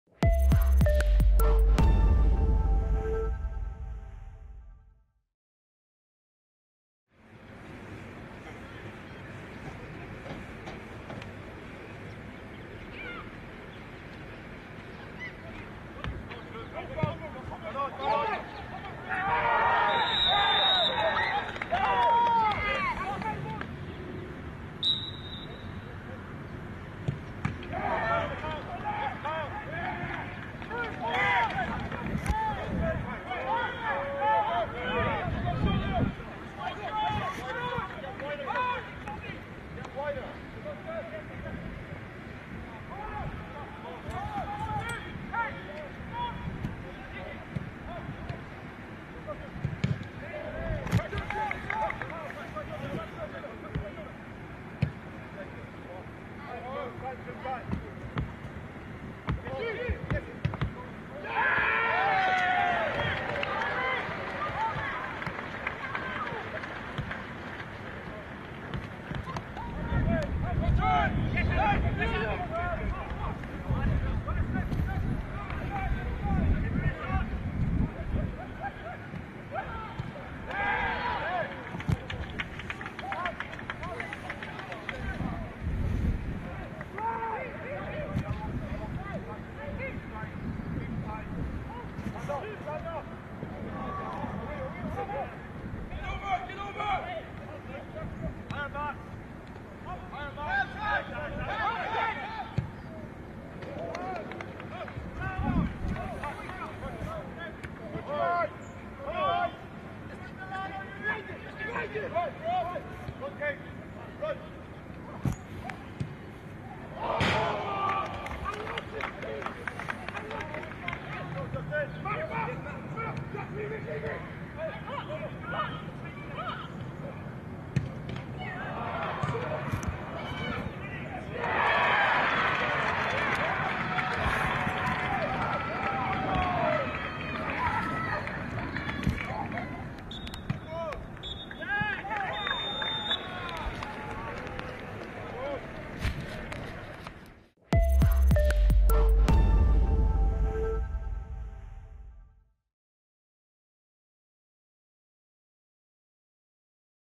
ACADEMY HIGHLIGHTS | Fulham U21 2-1 Man City U21 | Our Winning Start Continues!
Take a look at all the key moments as Fulham Under-21s hosted reigning champions Manchester City.